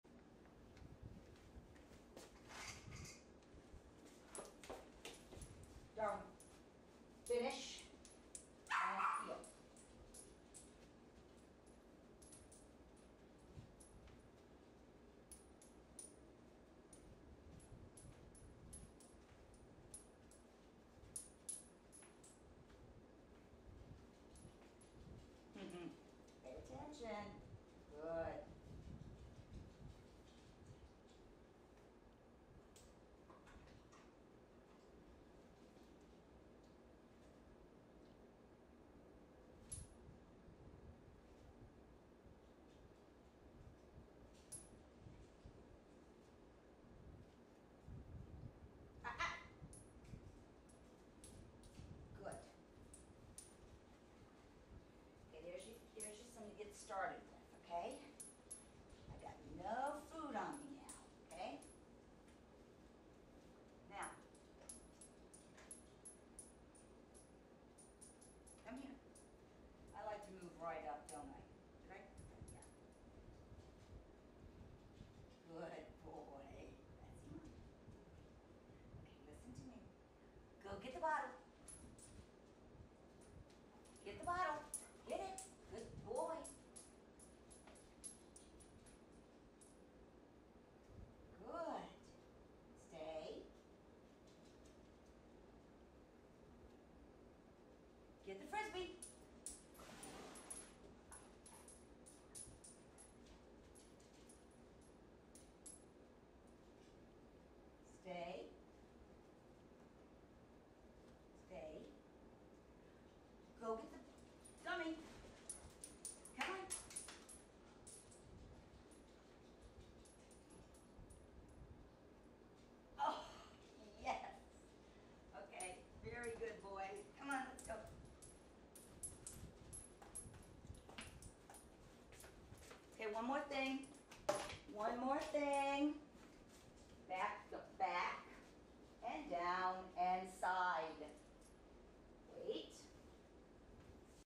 Darwin 1st attempt retrieving three objects
We've worked a couple of days using three objects (generalization), retrieved individually and reinforced, today I ratcheted things up to see IF Darwin could/would retrieve three objects before reinforcement.